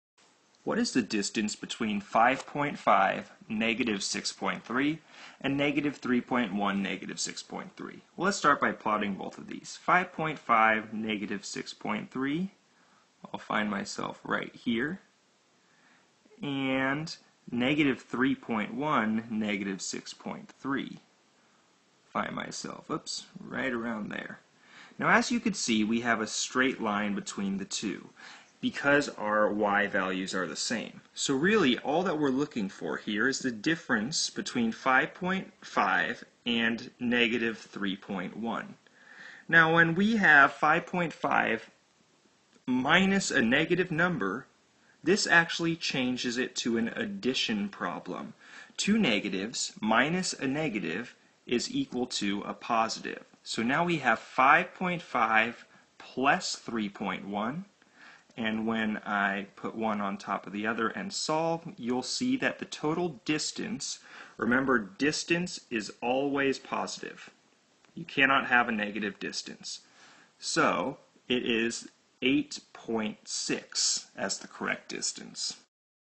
[6.NS.8-2.0] Graphing Points in Coordinate Plane - Common Core Standard
Use coordinates and absolute value to find distances between points with the same first coordinate or the same second coordinate.